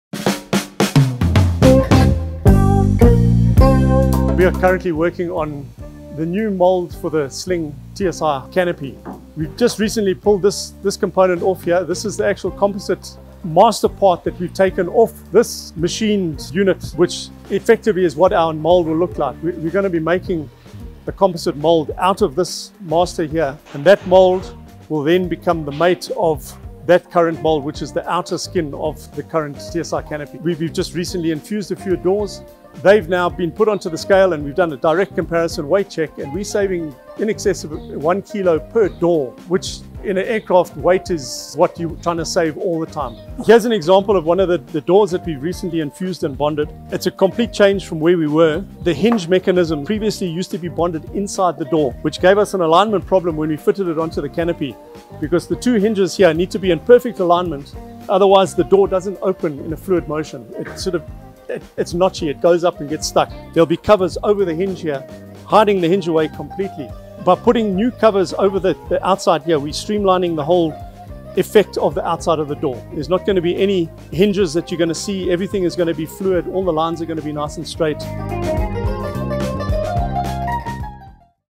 Our New Sling TSi Canopy (Part 2)
Some of you Slingers might remember a few months back, in our March edition of the Sling Squawk, we discussed our investment in Resin Infusion Technology in our High Wing models and how thanks to the significant improvements in quality, we have decided to invest in new moulds for our Sling TSi canopy, undercarriage and wing tips that allow us to resin infuse them as well.  Fast-forward 8 months and we have now perfected this process.

In part 2, we learn how this infusion process and bonding has so far saved 1 kg per door and of course, in an aircraft, weight is of vital importance! This is all in a bid to ensure faster production of simpler materials with stronger properties!